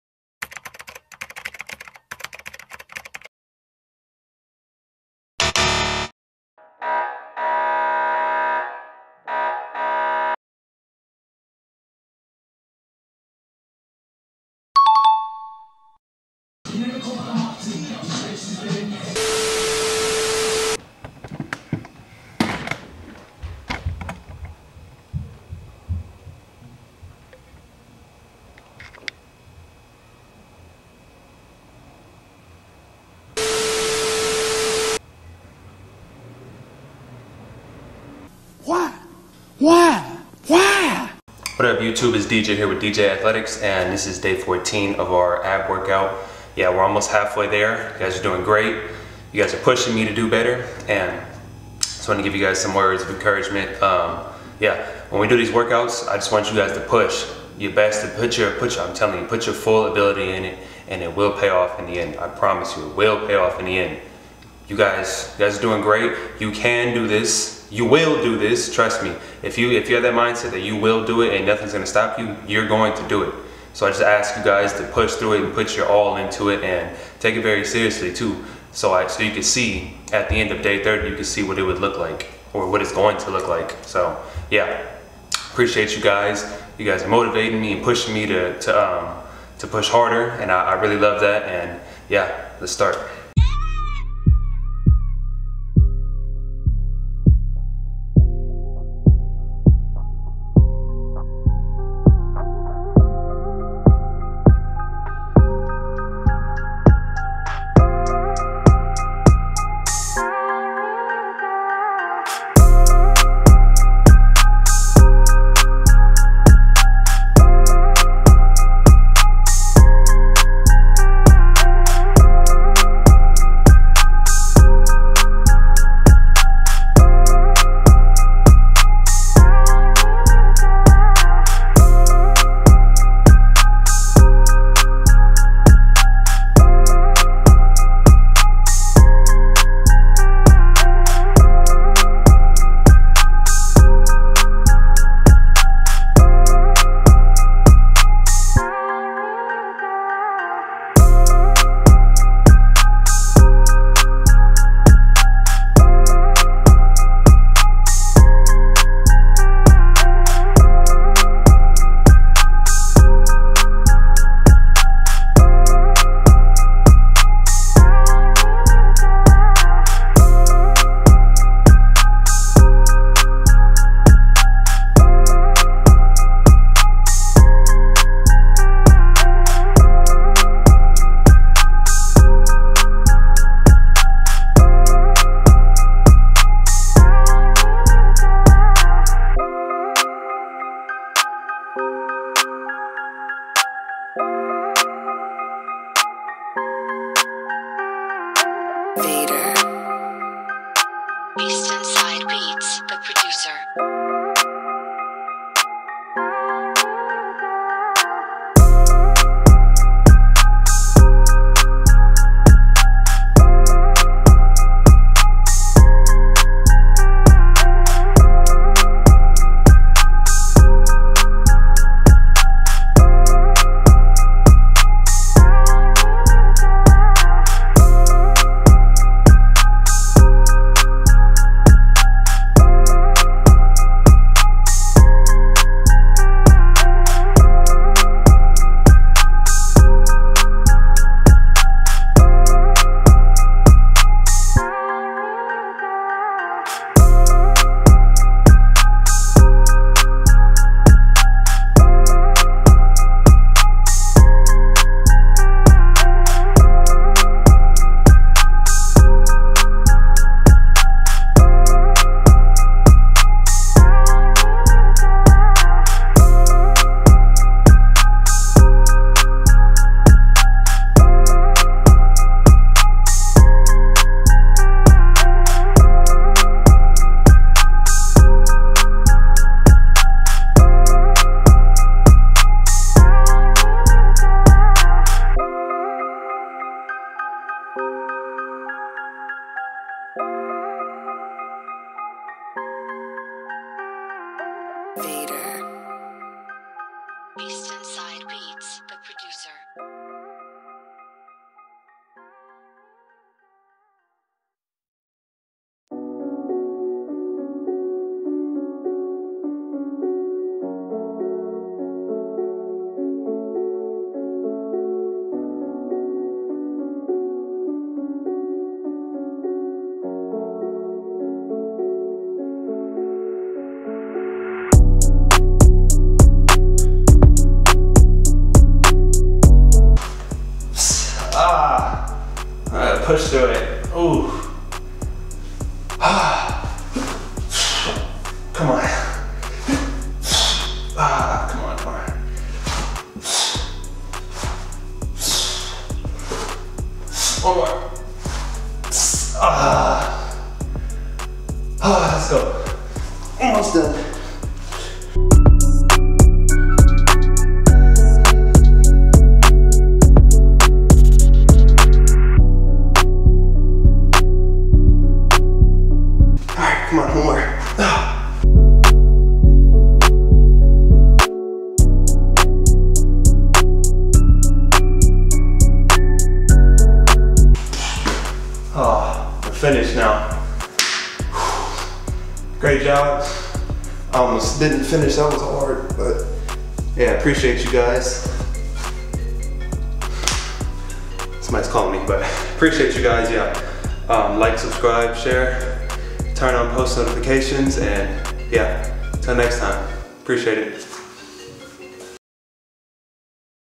How To Get A Six Pack (Day 14)
Day 14 of our 30-Day journey. Congrats for making it this far. You are doing great. Day 16 we are going to add a jump rope. So I'd recommend you buy one if you don't already have one. Do it with me. You will do it. Keep pushing and don't give up.